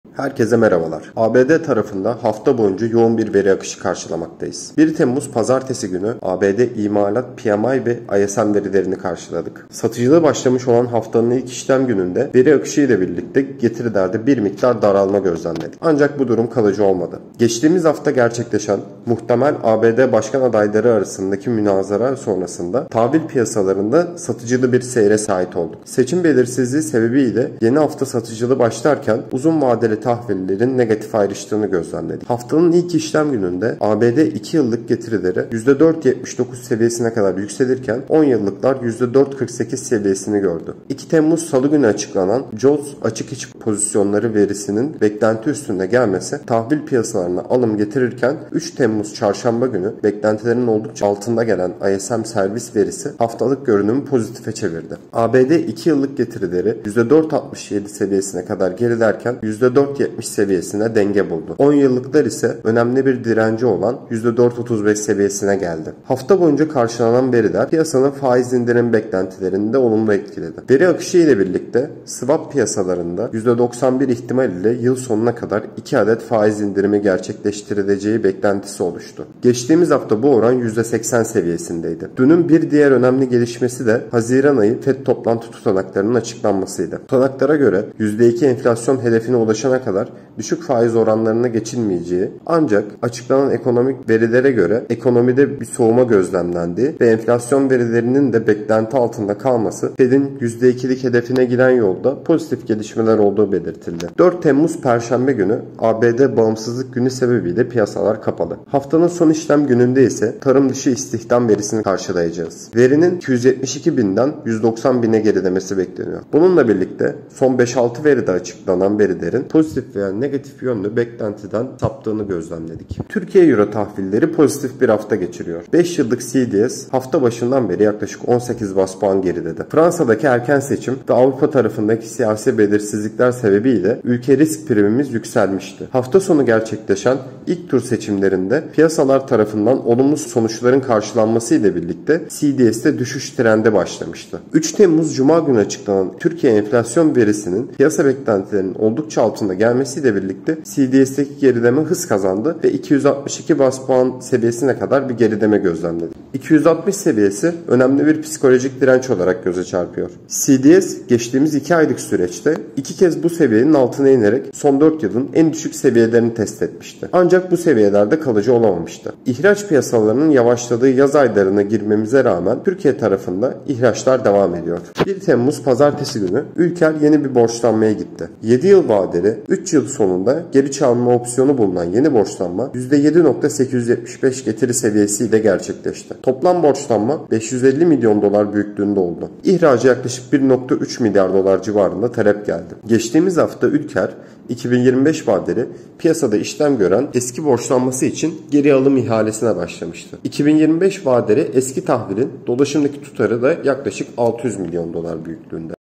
EUROTAHVİL (EUROBOND) PİYASASINDA NELER OLUYOR? - 04.07.2024 - BARIŞ YAVUZ
📌 İş Yatırım Uluslararası Piyasalar SGMK ve FX Müdürlüğünde Barış Yavuz, Eurotahvil (Eurobond) piyasasında Türk hazine kıymetleri, ABD hazine tahvilleri ve gelişmekte olan ülkelere dair analizlerini paylaşıyor.